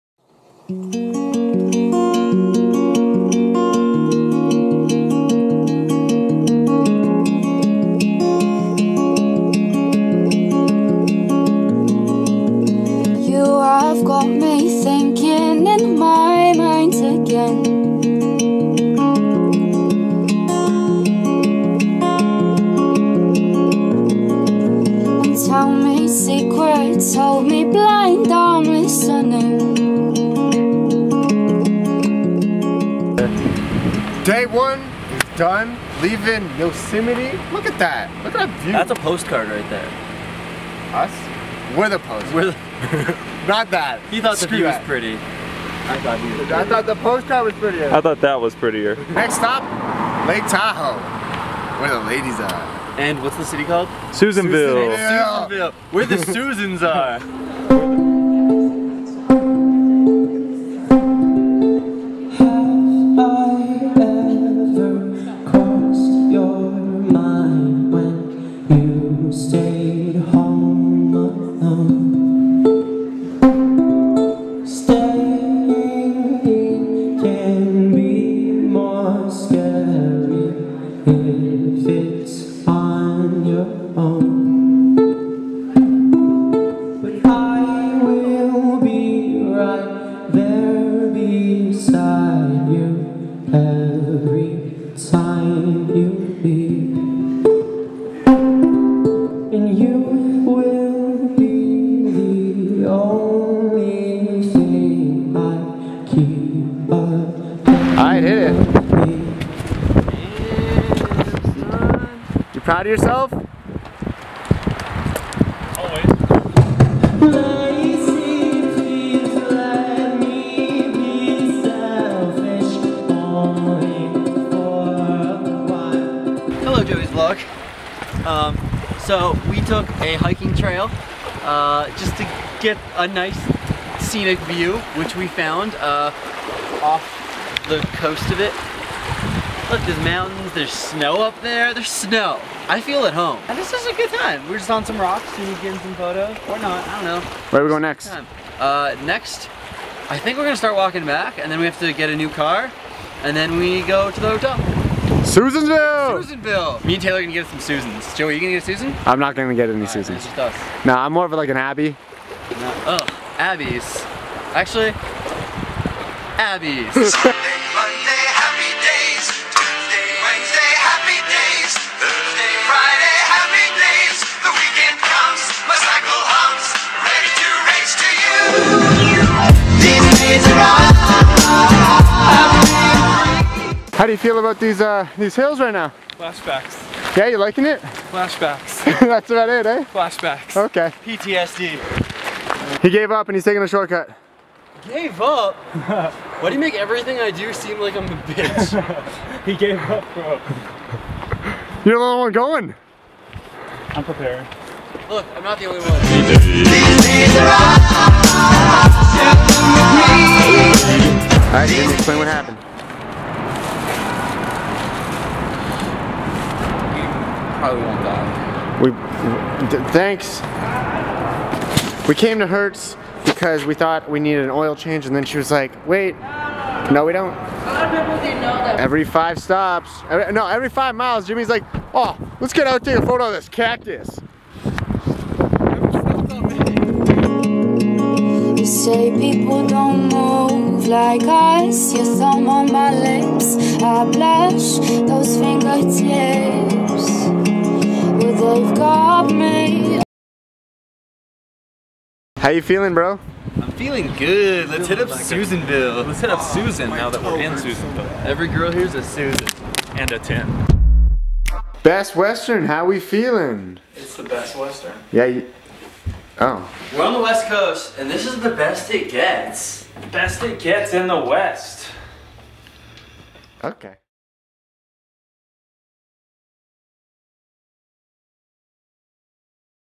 ROADHEADERS DAY 2 // LAKE TAHOE
This is Day two of the road headers journey across the west coast. We stopped in at Lake Tahoe and then crashed in Susanville for the evening.

FREE STUFF:
Uber Eats Code: Eats-UberJoeyKidney
Uber Code: UberJoeyKidney